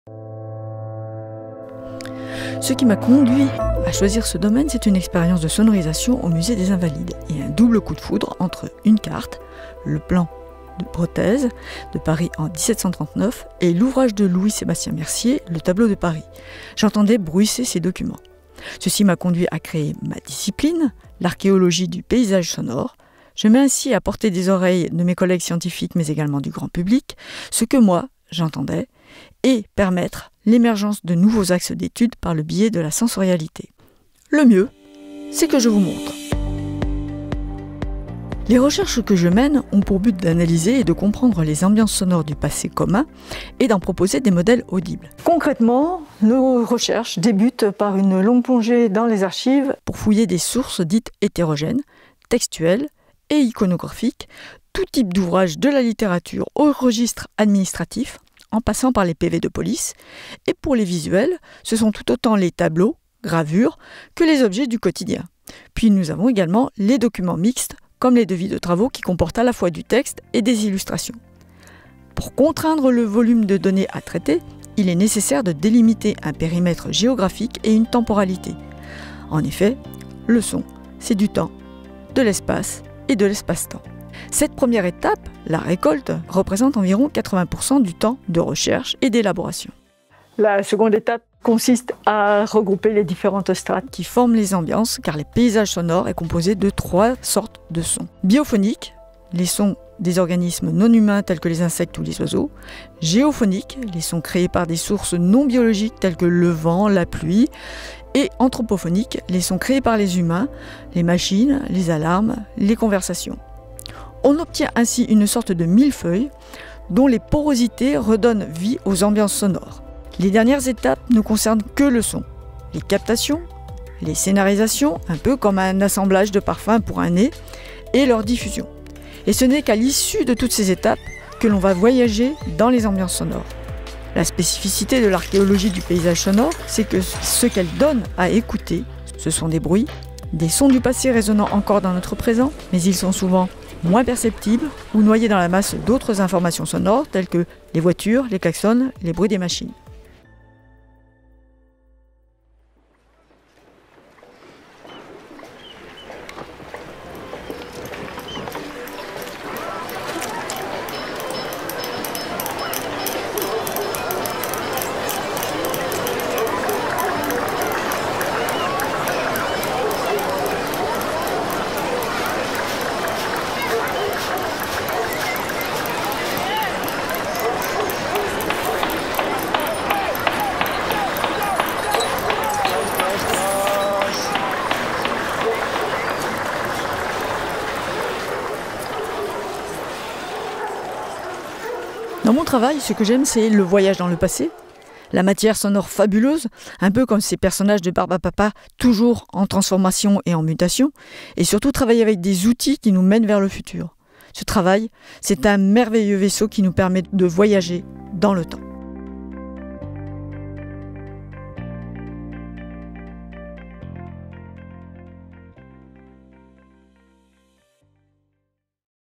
Mylène Pardoën, ingénieure de recherche en paysage sonore | Talents CNRS
Archéologue du paysage sonore à la Maison des sciences de l'Homme Lyon Saint-Étienne, créatrice et coordinatrice des projets Bretez et NOMADHISS, experte scientifique pour la restauration de Notre-Dame.